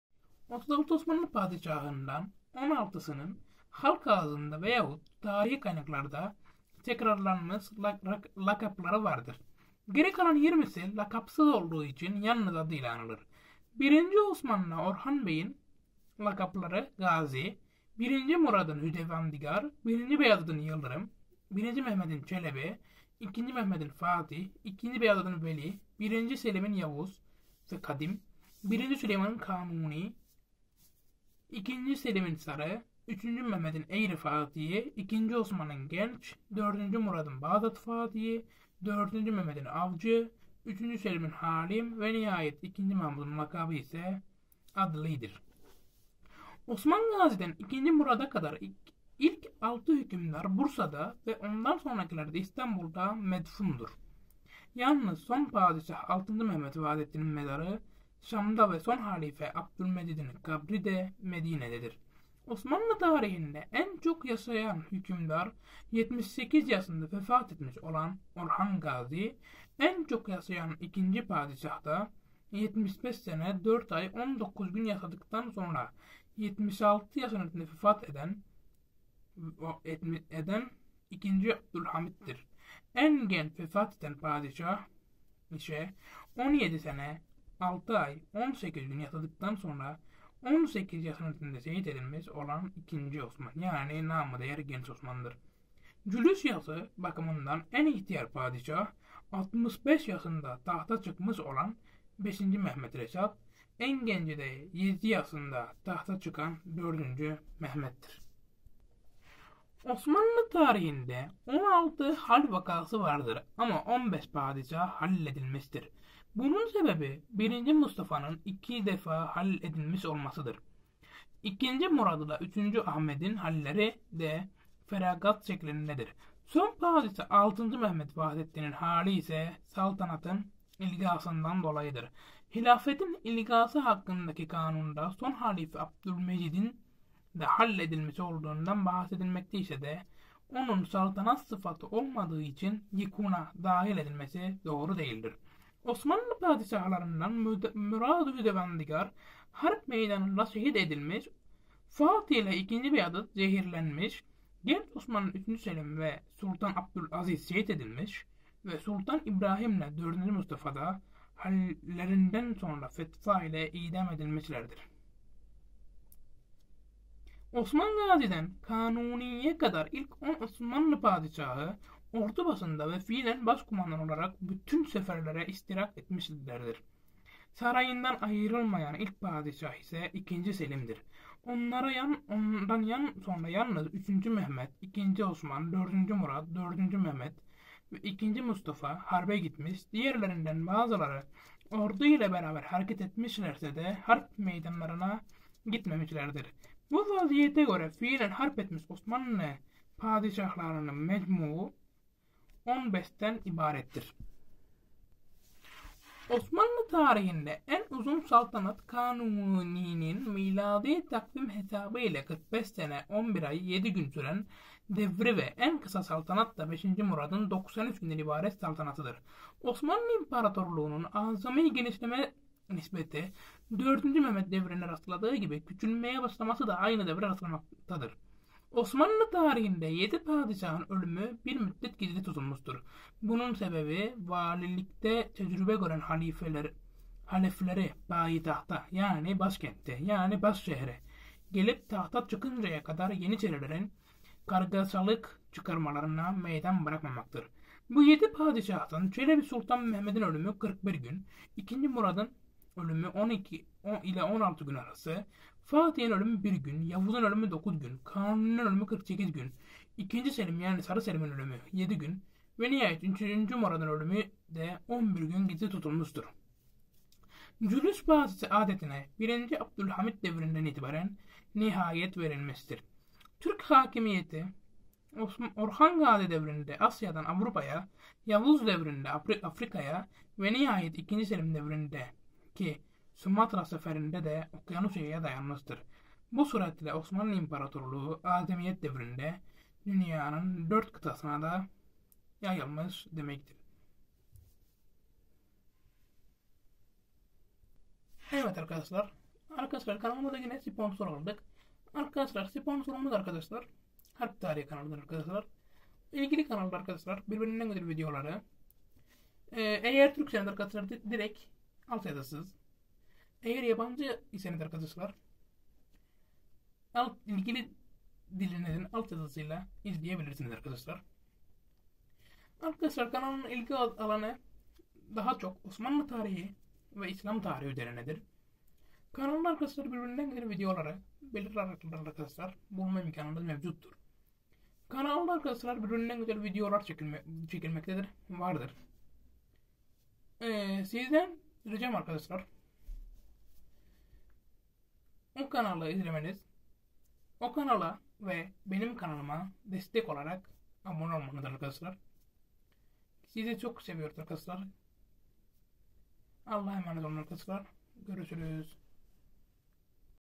OSMANLI İMPARATORLUĞU HAKKINDA ÖZEL BİLGİLER
BU VİDEOMUZDA :

OSMANLI DEVLETİ İLE İLGİLİ BAZI ÖZEL BİLGİLERİ ÖĞRENECEĞİZ